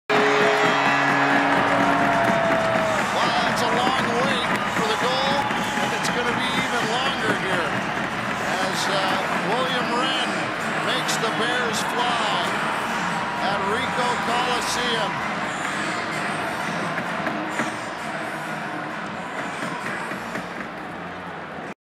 Teddy Bear Toss - December 3, 2016
Watch the Teddy Bear Toss commence at Ricoh Coliseum following the goal from William Wrenn.